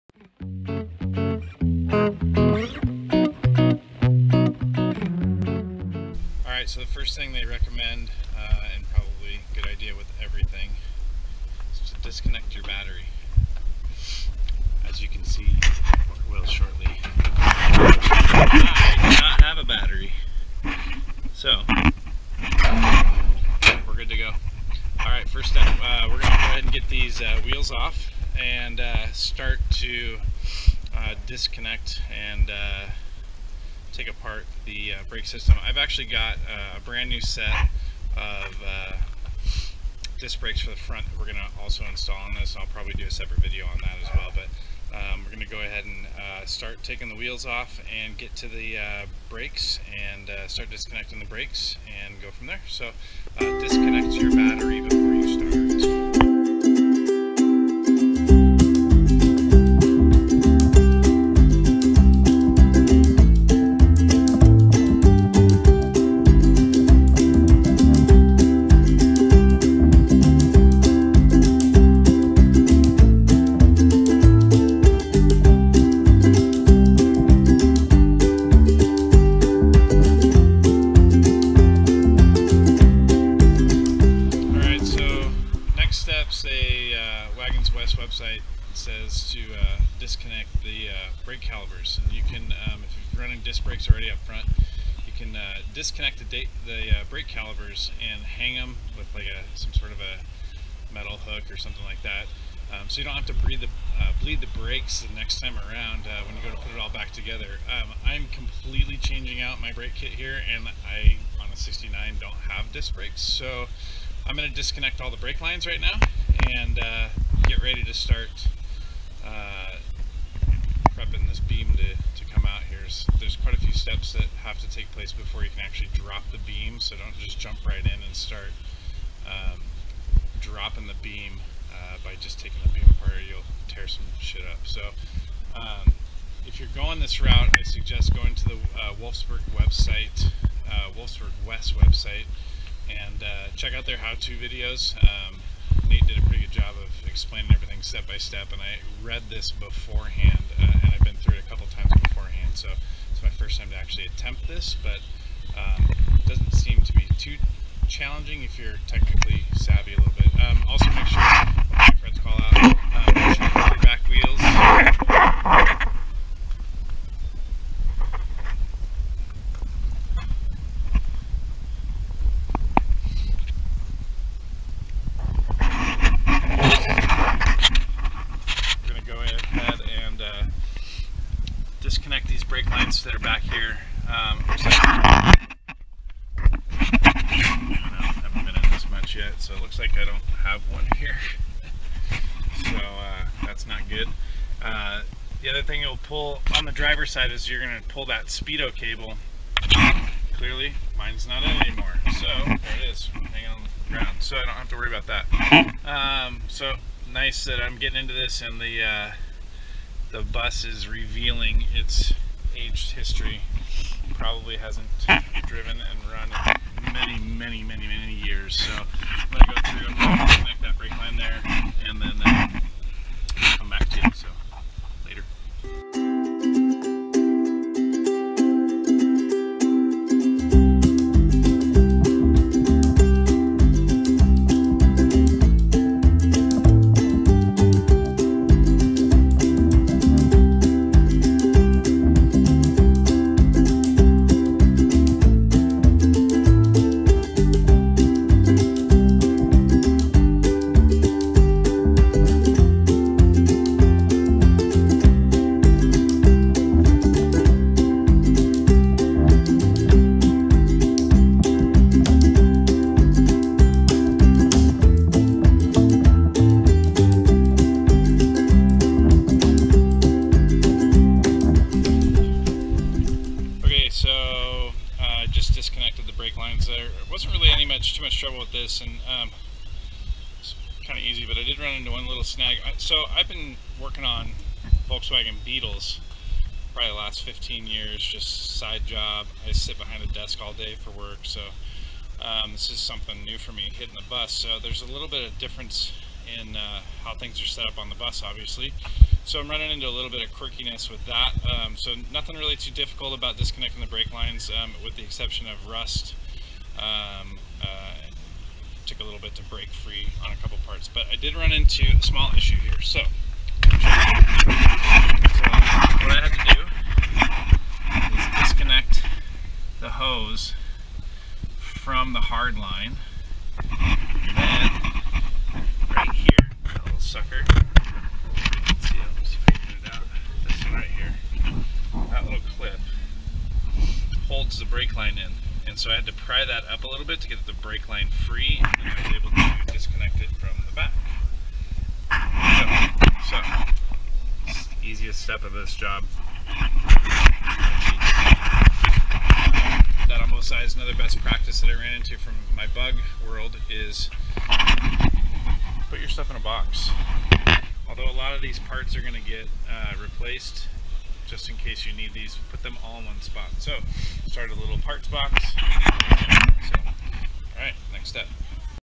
WagensWest Slam Bay Kit Install Pt.1
Part 1 of my 1969 Baywindow Bus WagensWest Slam Kit Install.

This video covers front brake prep to start the stock beam removal.

Purchase the Kit HERE!